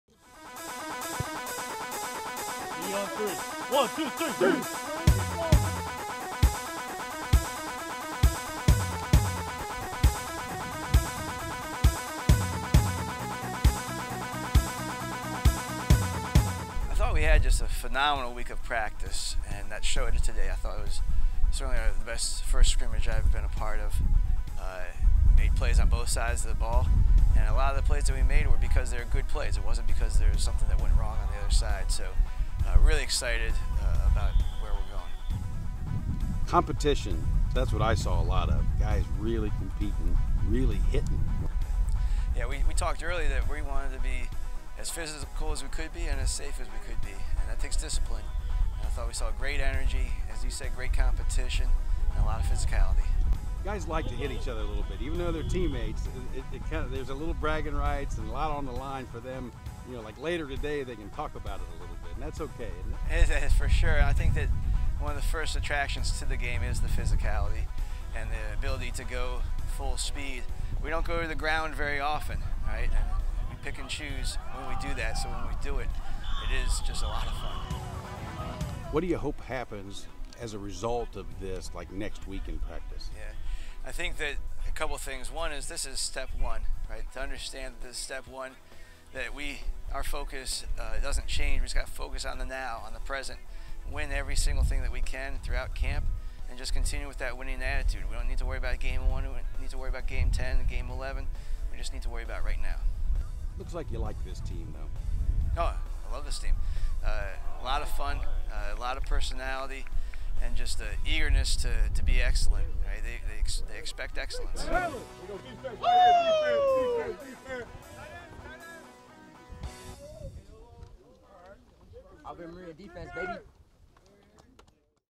AMU Scrimmage 081421
After a great first week of camp, Coach Joe Patterson and staff turns the Gyrenes loose in the first scrimmage of 2021.